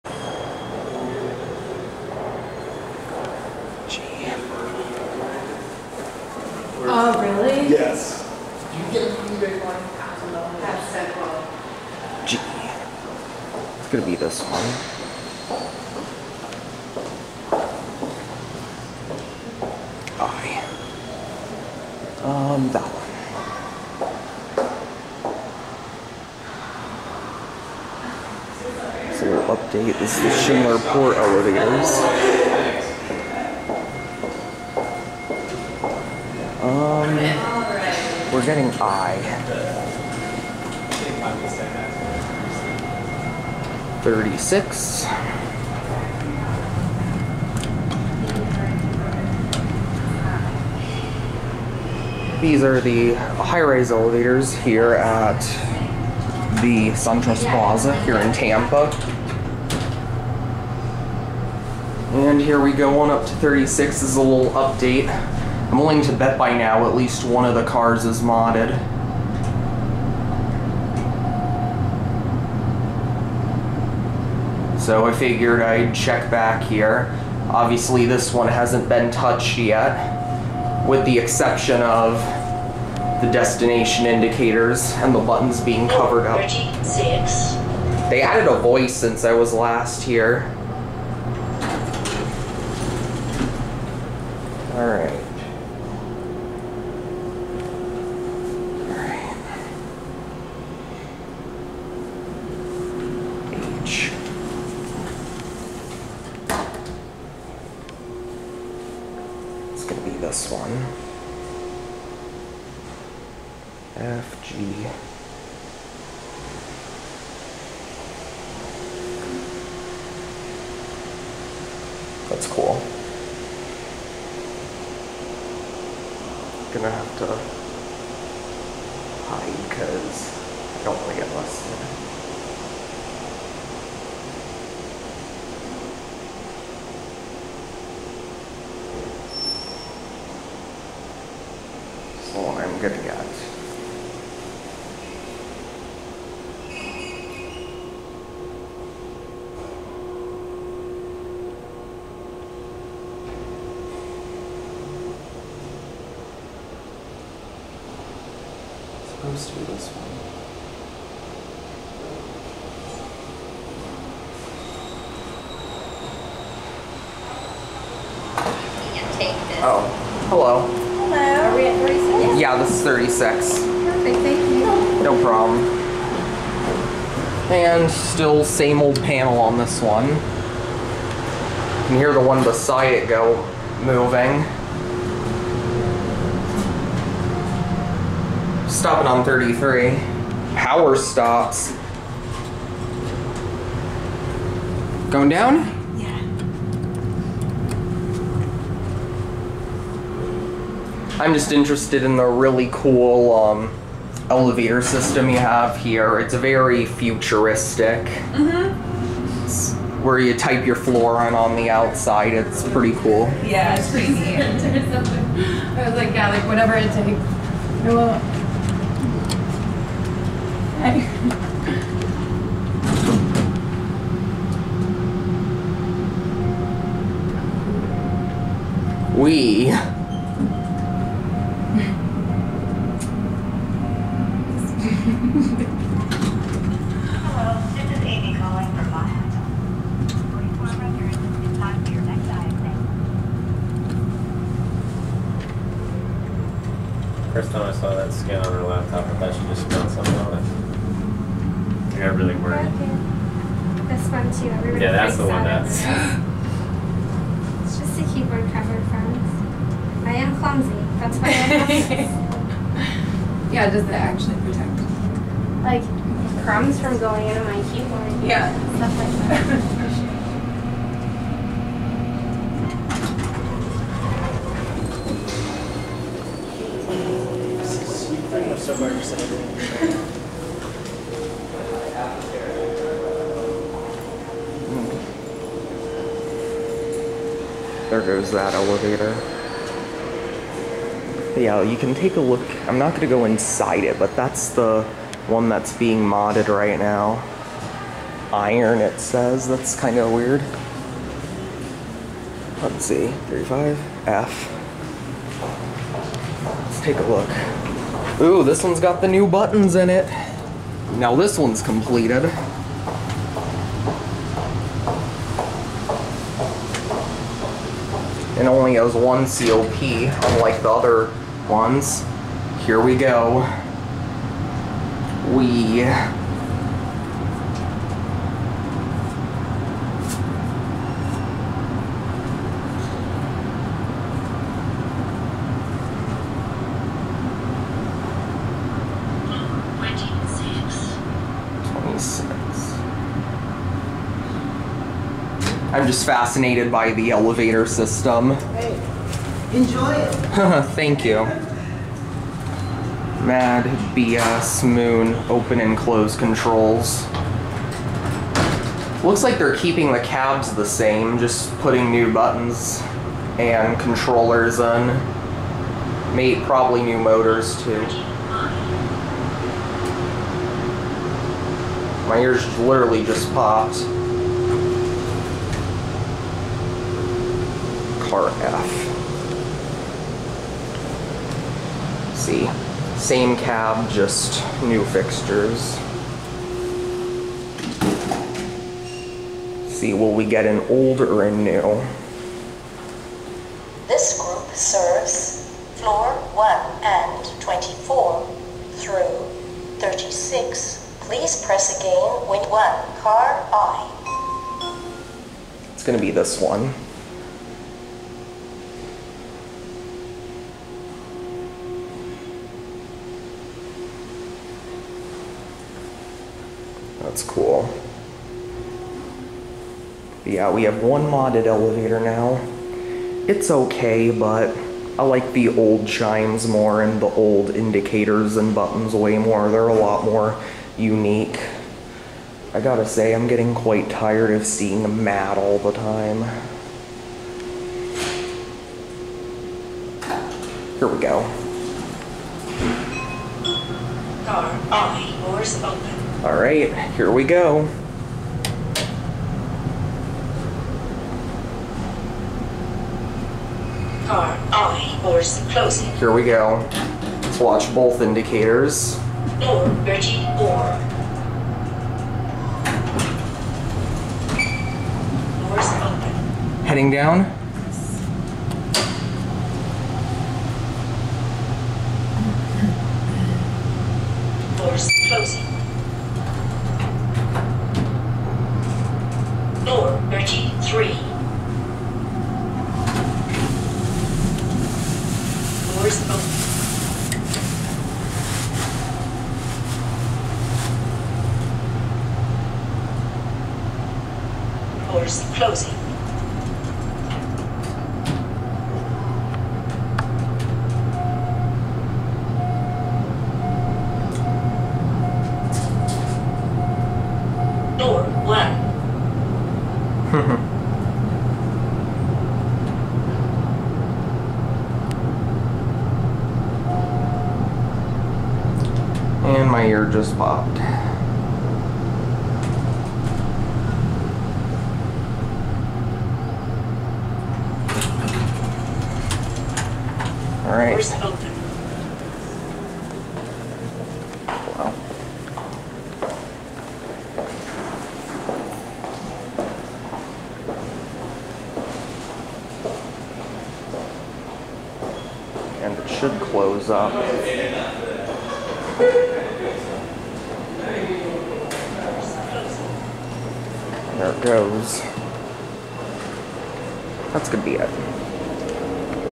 Car F is done, Voices Added! Update on the Schindler PORT Elevators at SunTrust Centre in Tampa, FL
Filmed in March of 2020.

I figured since today is Ronin's (Elevating Chicago) Birthday, and knowing that his favorite elevators are anything that is High Rising, I figured I'd upload this exciting new update taken in Early March regarding the Schindler PORT Elevator Modernization currently ongoing at SunTrust Centre in Tampa.
As you guys may remember, these elevators were Originally Schindler with Unique Custom EPCO Fixtures, then in May of 2019, Schindler started modding these, and I filmed my first video of these with the New PORT Panels in use in September, but at the time, none of the cars were modded yet with the exceptions being temporary DD indicators and the floor buttons being covered up.

Well, Fast Forward to March of 2020, at least one elevator in both the High Rise & Low Rise Banks is fully modded (Car F in the high rise) with New MAD BS Moon Door Controls, Blue Indicators, New COPs as a whole, New Controllers and possibly new motors. The Cab on the Newly Completed Car F was still the same though, and even though most of the cars still have their original COPs and equipment at the moment, they all have BRAND NEW Schindler Voices on them! (At least for the most part anyways)

The Cars that still have their original COPs and motors, equipment and chimes are an absolute TON of fun to ride, and will most certainly be missed when they're gone.
As for the mod, I like the New and Improved PORT Destination Dispatch Panel and the New UI a LOT, and it is a DEFINITE Improvement over the Older PORT UI, and the Destination Dispatch system has been doing its' job very well so far.
But in terms of the elevator cab mod, I really am not too impressed... They only installed ONE COP in the New Car, the Cabs are the same which while nice, the New COP just looks SUPER Generic and unlike the Old Elevators, the indicator and voice are totally out of sync from where the elevator actually is. It just seems that are far as the actual mod, they are really cheaping out on it.

On the positive side, the speed is the exact same, and they're still a lot of fun to ride, but the Original Cars will most certainly be missed.
And for those wondering, the parking elevators have just started their mod.

Anyways, with that all being said, I hope you all enjoy this New Update on the Current State of this BRAND NEW Schindler PORT Elevator Modernization (The FIRST PORT In Central Florida), and Have a VERY Happy Birthday Ronin!!! Enjoy!

Installed in 1993. (Schindler Traction with Custom EPCO WA? fixtures and Circular Indicators with RT/MT Style floor passing Chimes and Asian style lantern chimes)

Being Modernized since 2019. (Schindler PORT Destination Dispatch with New Controllers, New MAD BS Moon COPs, New Letter Lanterns, DD Indicators, Possibly New Motors, etc. The Cabs are being left original.)